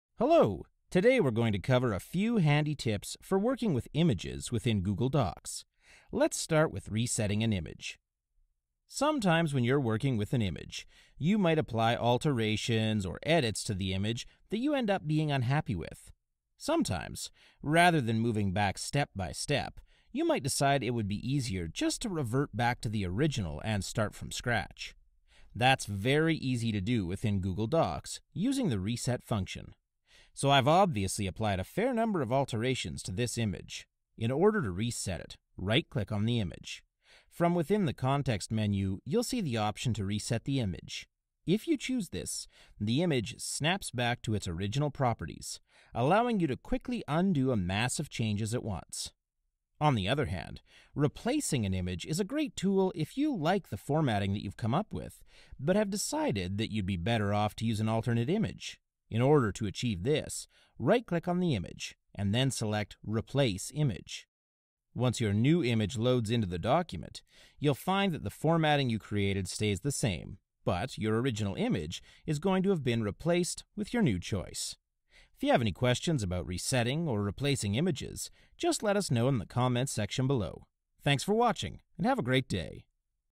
How to Reset and Replace Images in Google Docs
When you’re working with images in Google Docs, there are a few useful functions, like “Reset Image” and “Replace Image.” Here are some tips for when and how to use them.